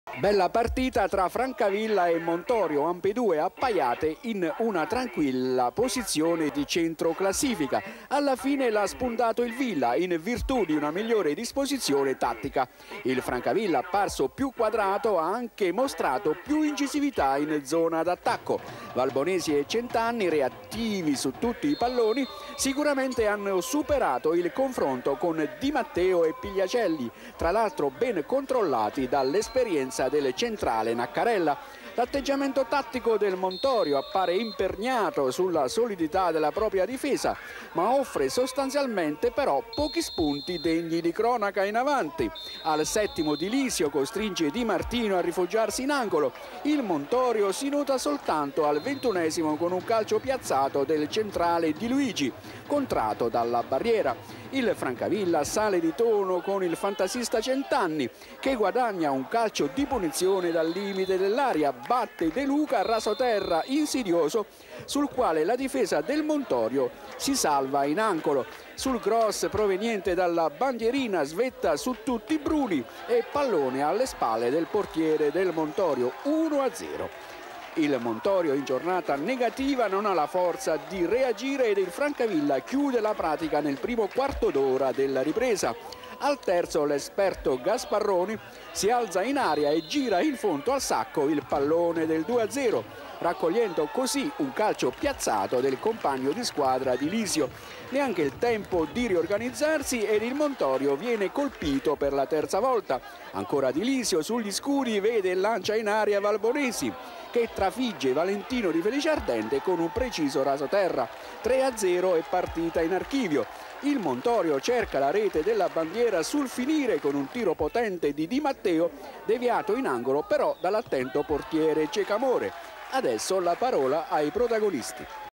Francavilla Montorio 88 3 0 13 11 2005
Calcio d’archivio (Eccellenza): Francavilla-Montorio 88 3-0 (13/11/2005)